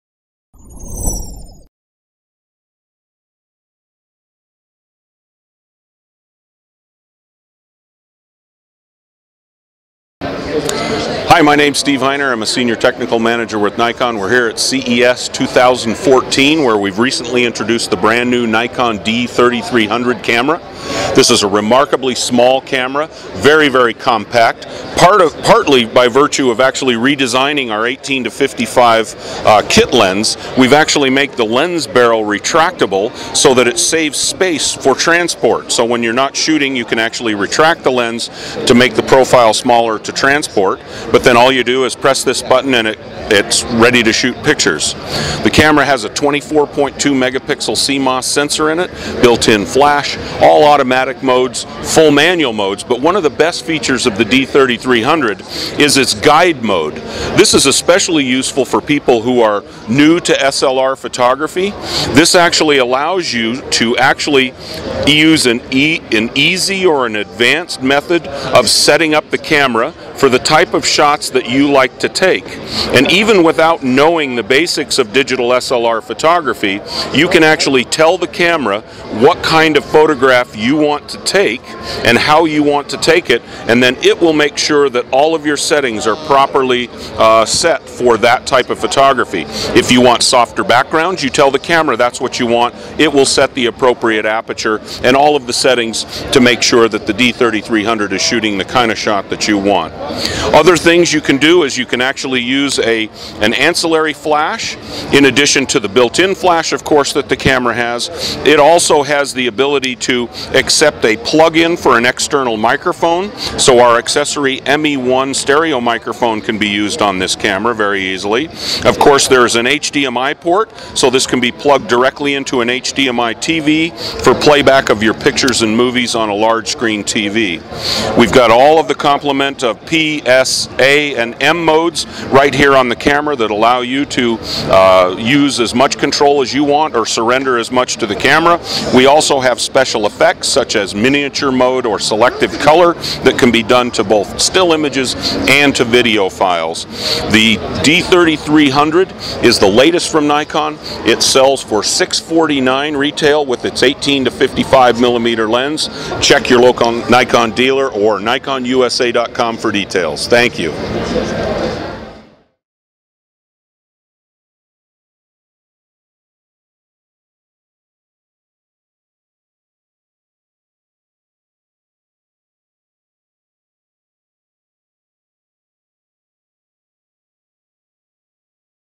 CES 2014 | Nikon D3300 DSLR Demonstration | Latest Digital SLR | Smart Review
SmartReview.com attended the Consumer Electronics Show (CES 2014)  and saw the newly introduced Nikon D3300 Digital SLR. A new  AF-S DX NIKKOR 18-55mm f/3.5-5.6G VR II lens is ultra compact for travel.  The D3300 DSLR features 24.2 megapixels, Full HD 1080p video, and an upgraded Expeed 4 image processor. High speed 5fps burst mode, and stereo microphone built-in. 1080p video and microphone input.   It is a great entry level digital SLR loaded with features borrowed from more expensive models. Product Demonstration by Steve Heiner, Sr. Technical Manager with Nikon USA.